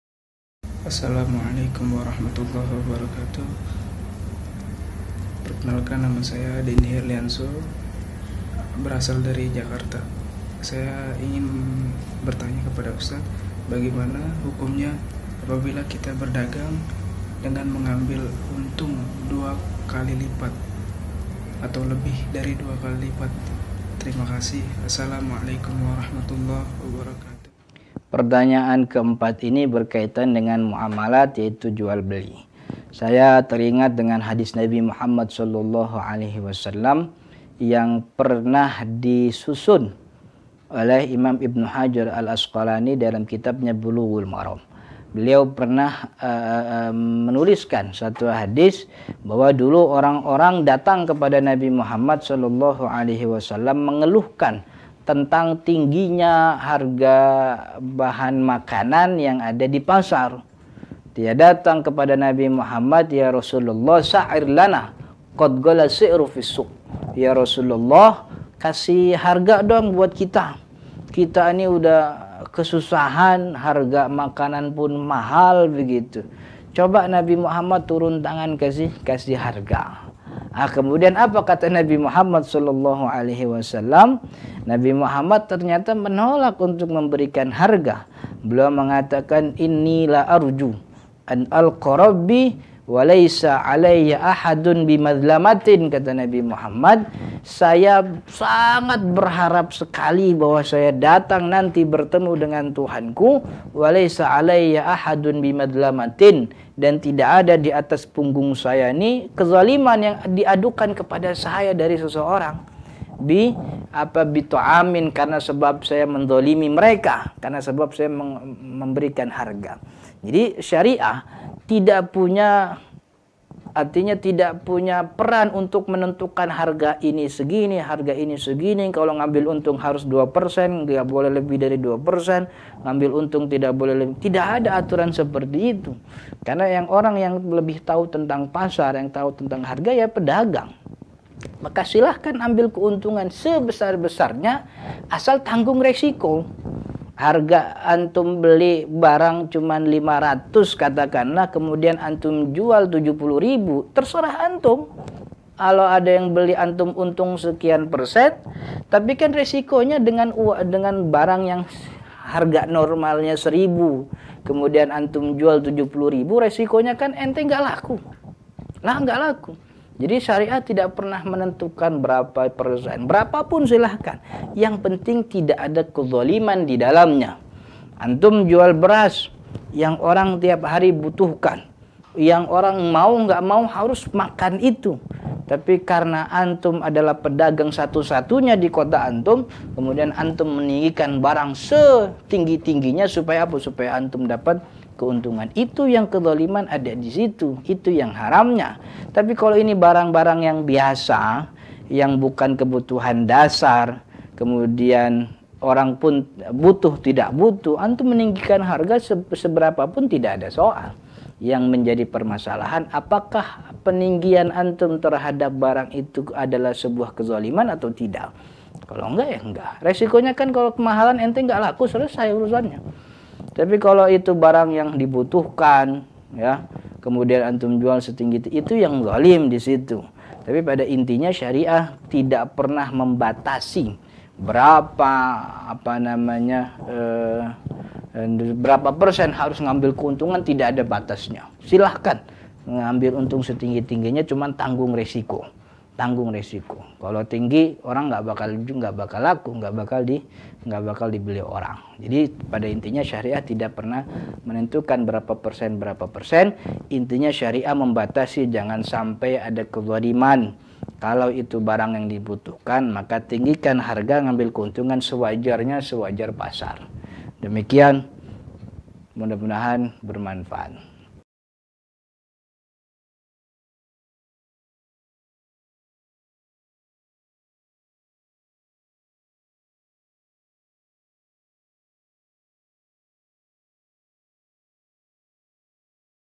"Hukum dalam Menetapkan Keuntungan Dagang" Ustadz Ahmad Zarkasih, Lc
Bekerjasama dengan Rumah Fiqih Indonesia.

Yas'alunak artinya Mereka Bertanya Kepada Mu.

Share Channel tayang di Palapa D, Symbolrate 7200, Frekuensi 4014, Polarisasi Horizontal.